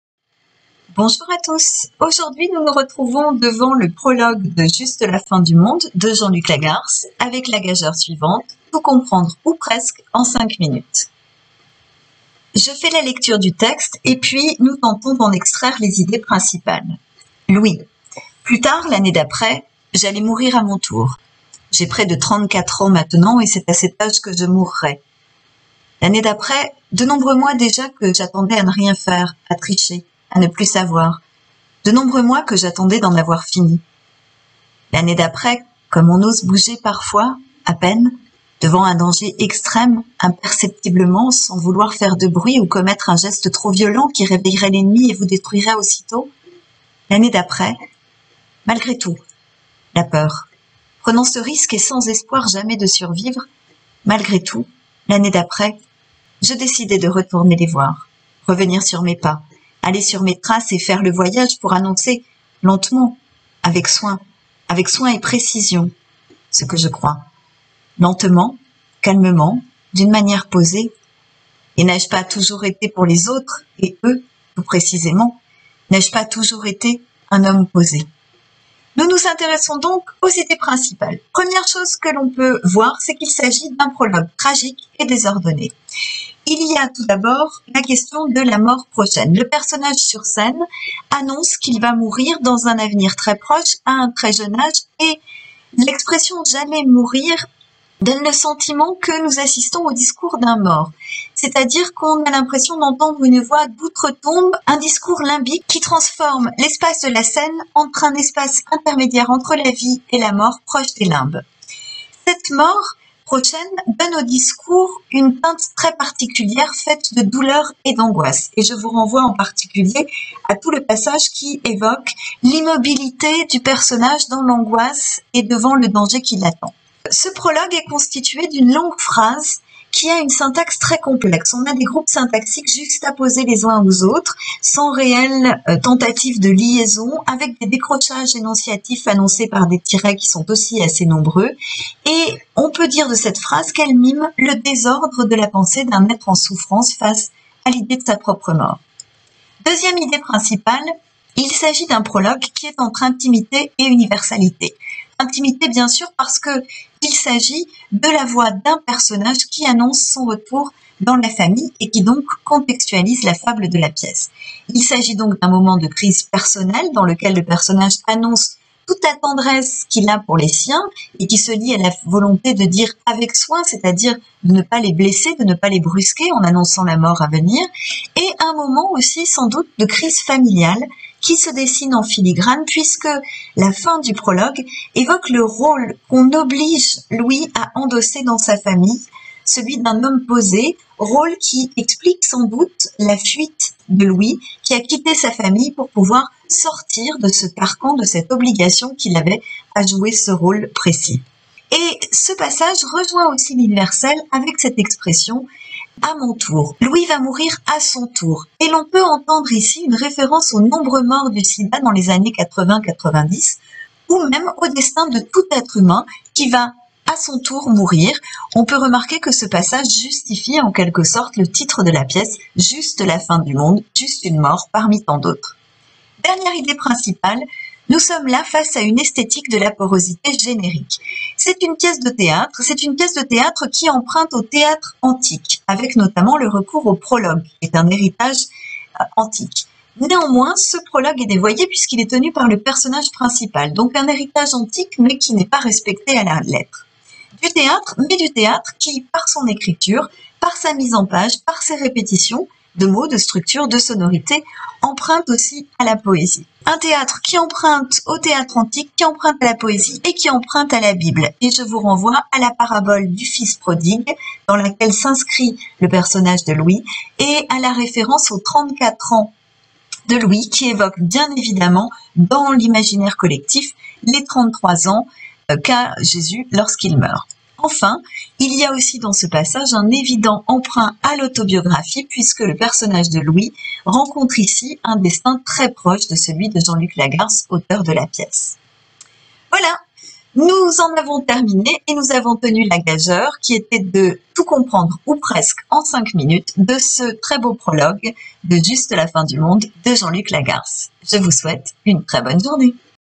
Prologue, Juste la fin du monde, Jean-Luc Lagarce - TOUT COMPRENDRE EN 5 MINUTES !!!
Une "fiche de révision" qui s'empare du "prologue" de Juste la fin du monde (Lagarce) et tente d'en extraire les principaux enjeux.